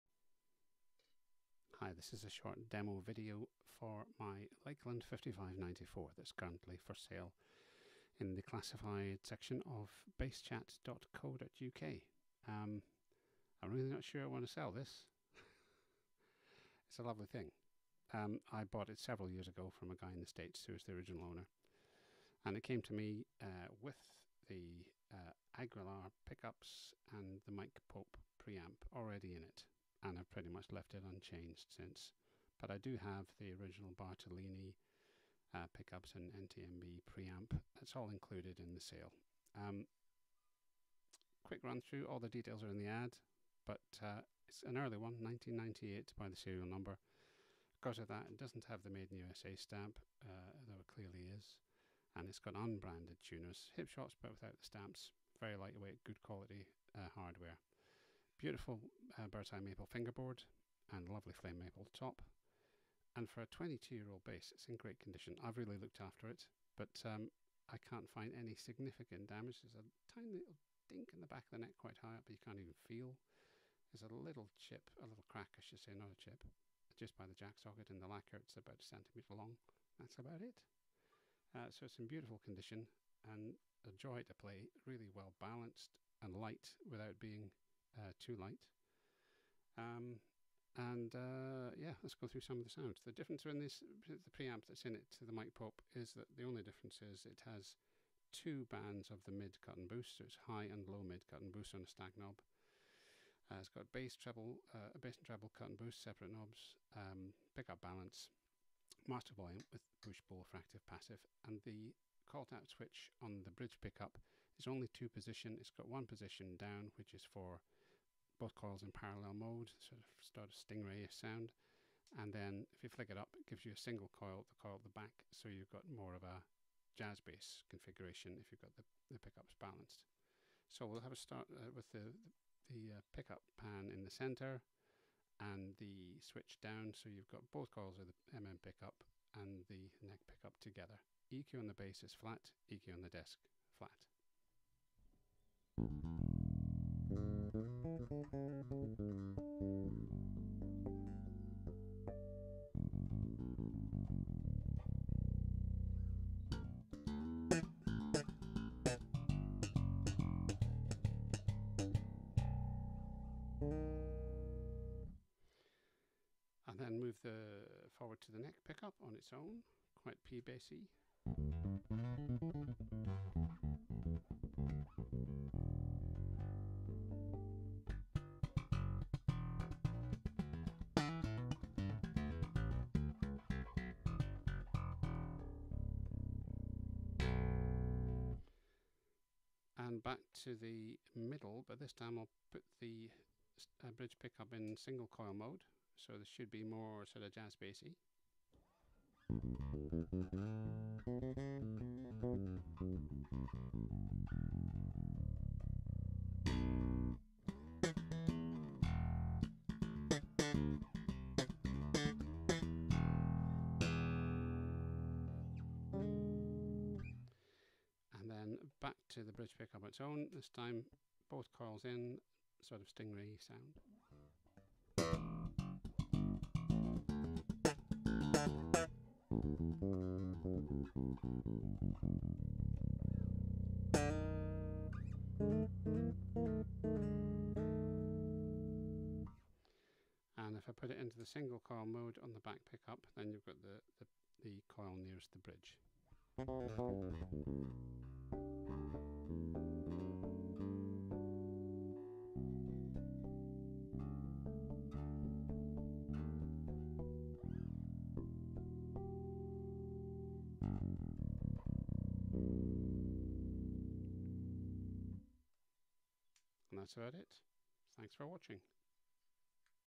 Lakland 55-94 Deluxe Bass Short Demo - Aguilar pickups, Mike Pope preamp
Short demo video of a Lakland 55-94 Deluxe fitted with Aguilar pickups and a Mike Pope preamp